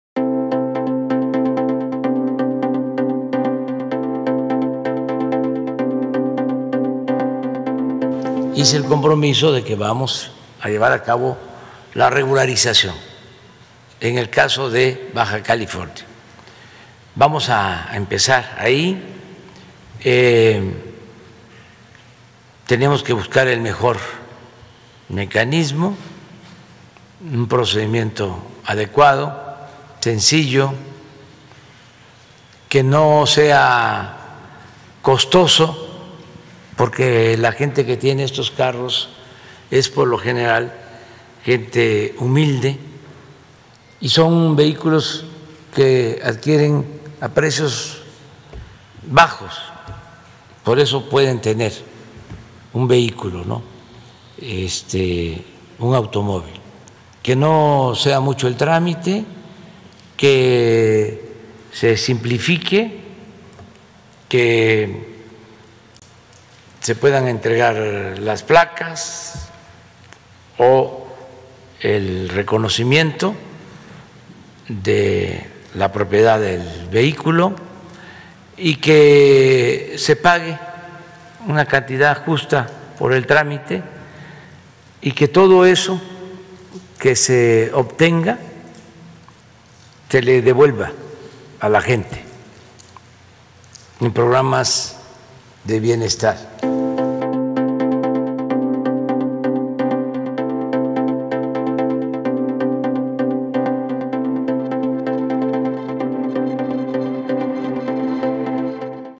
Buscarán llevar programa de regularización de "chuecos"
"Autos Chocolate" serán regularizados en Baja California para combatir el crimen